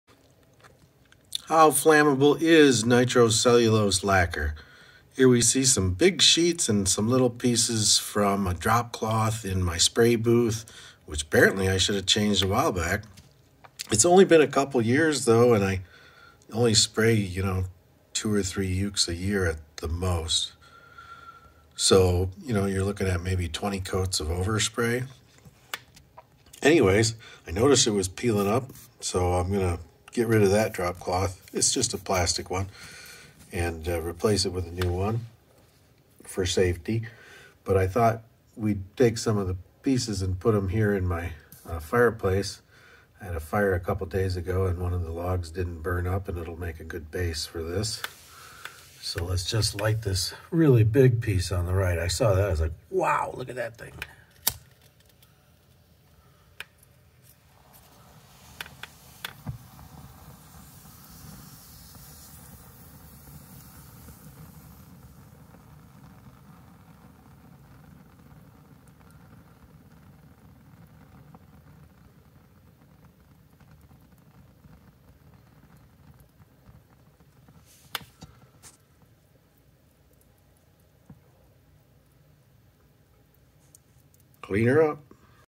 Just how flammable is nitrocellulose lacquer anyway?
Let's light up some large flakes of cured nitrocellulose lacquer and find out.